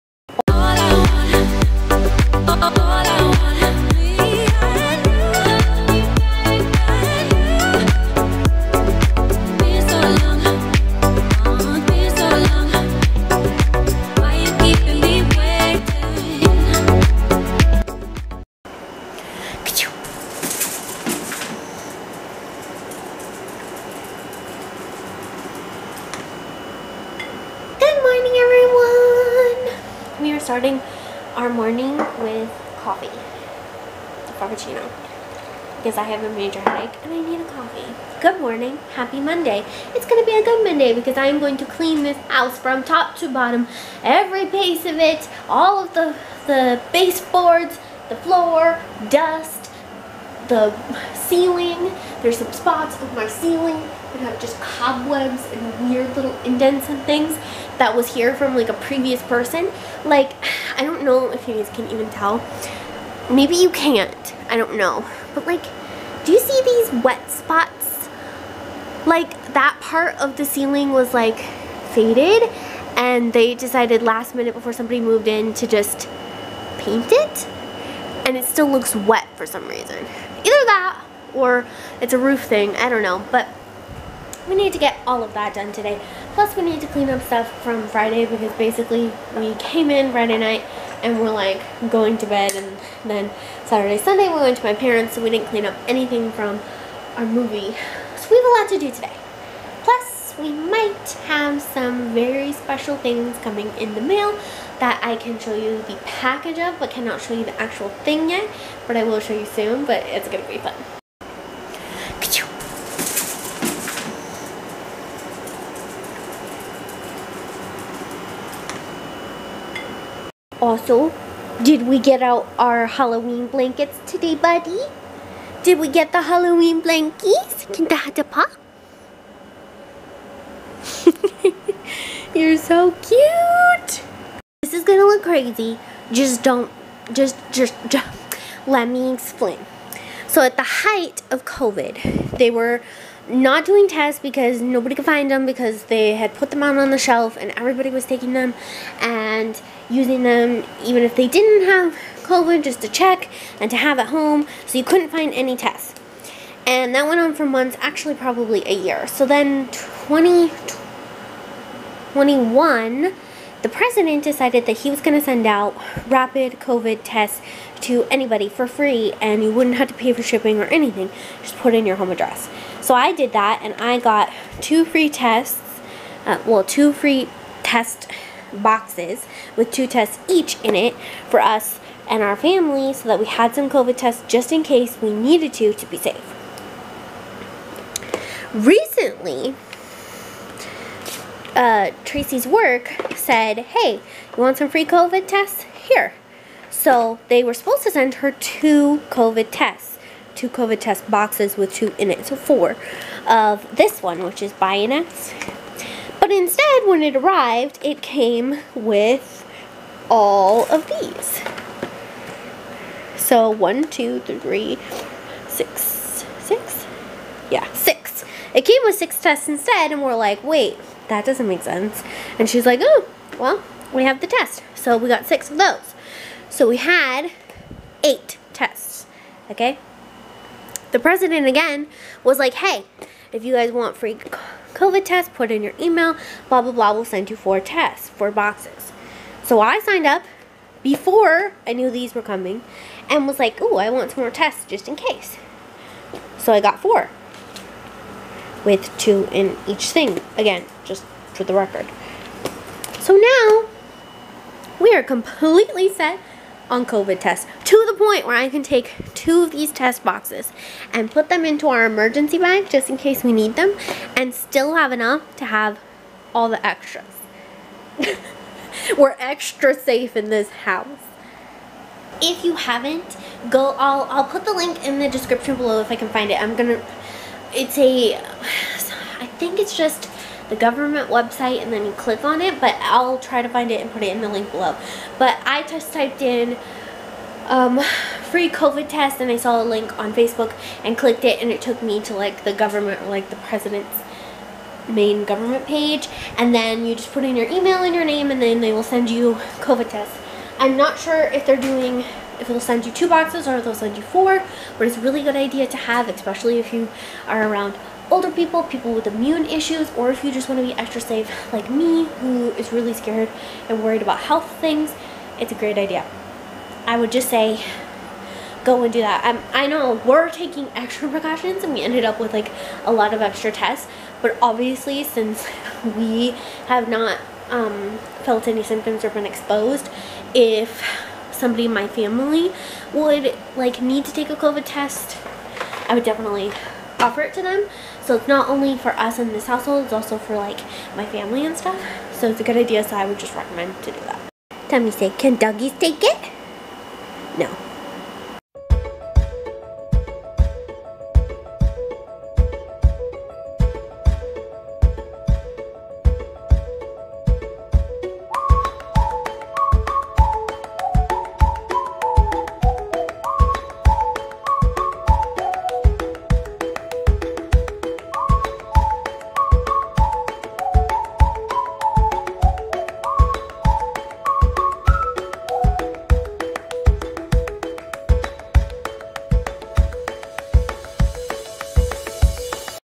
Ordered Covid Tests | Monday
|𝐒𝐢𝐦𝐢𝐥𝐚𝐫𝐬𝐢𝐦𝐢𝐛 𝐃𝐚𝐲 3344   𝐎𝐍 8.22.22   |
𝐈𝐧 𝐭𝐡𝐢𝐬 𝐯𝐢𝐝𝐞𝐨 we ordered some covid tests to be prepared and we see cute tommy playing

・❥・𝒮𝒾𝓂𝑜𝓃𝑒: @similarsimib
・❥・𝒜𝓃𝒹𝓇𝑒𝓌: @king_of_the_shades
・❥・𝒬𝓊𝑒𝑒𝓃𝒾𝑒: @queenietherobo
・❥・𝒯𝑜𝓂𝓂𝓎 𝟤 𝓈𝑜𝒸𝓀𝓈 : @tommy2socksthechi